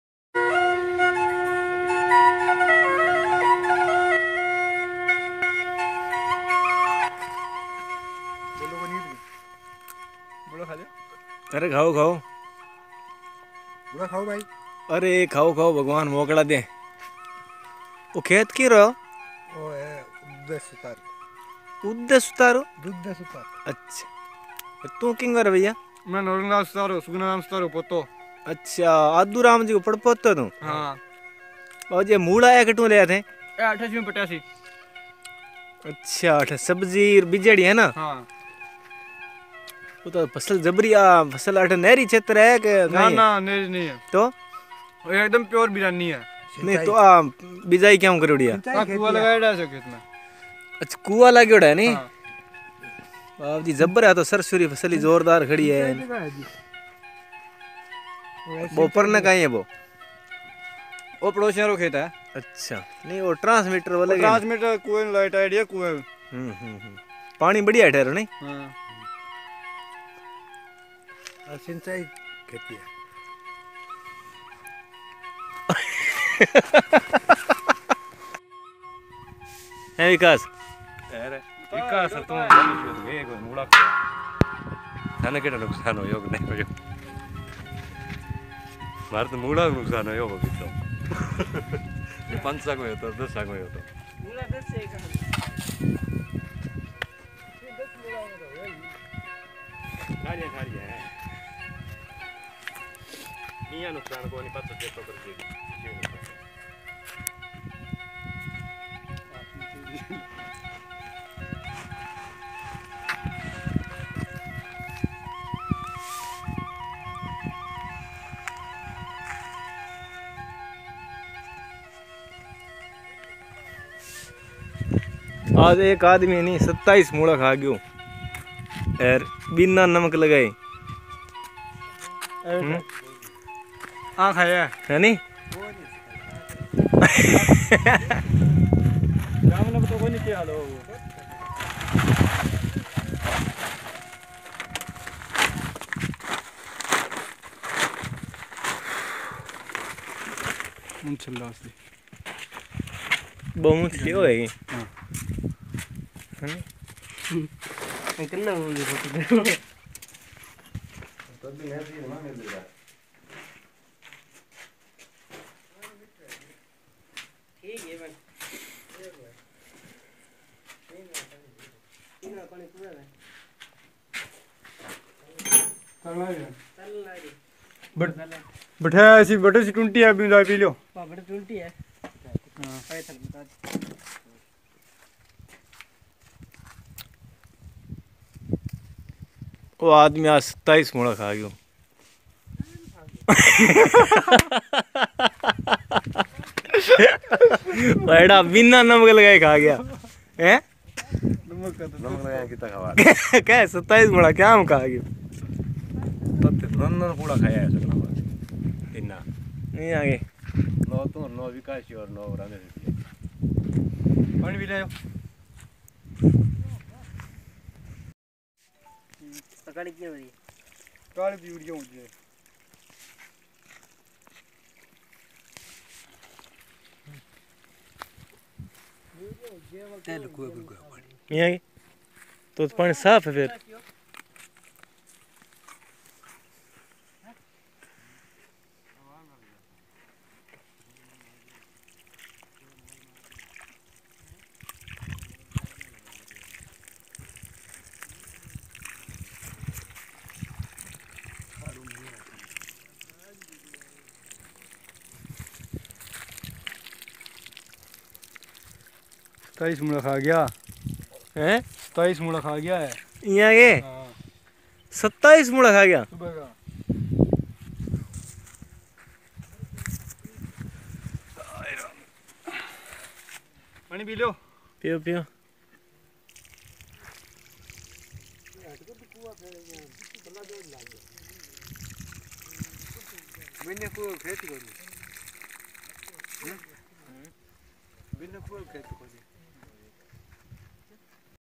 Cultivation of wells powered by solar energy | tillage crops | Dudaram Suthar Farm Udasar
Dudaram Suthar Farm Udasar VID_20200111_135440